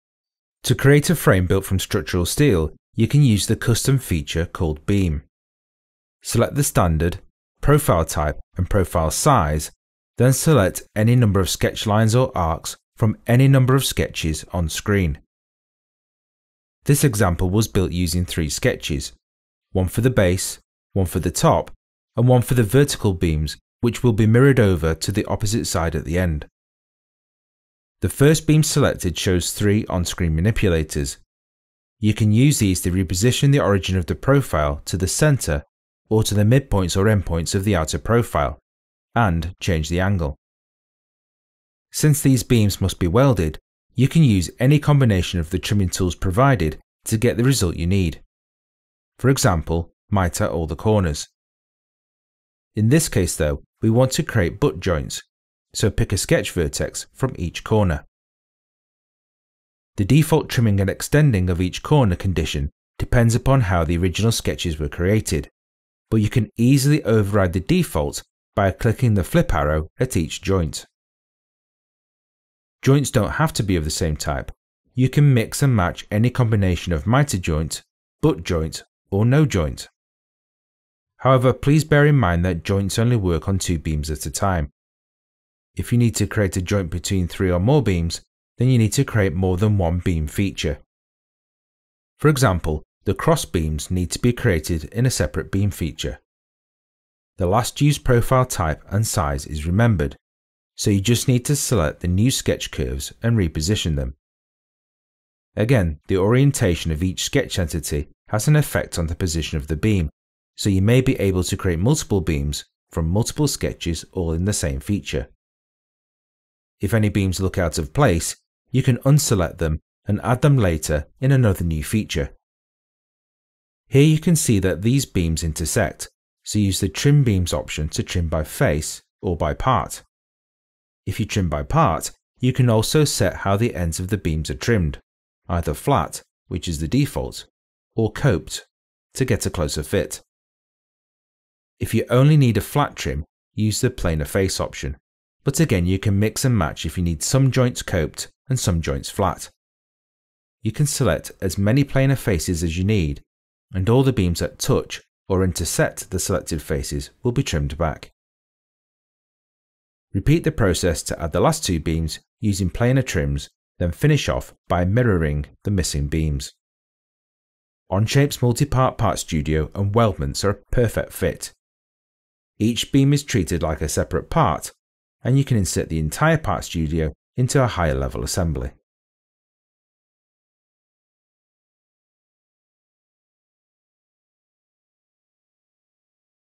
Beams - Custom Feature Spotlight | Tech Tip
Onshape’s custom Beam feature, available free on the FeatureScript Spotlight page, saves tremendous time on creating weldments.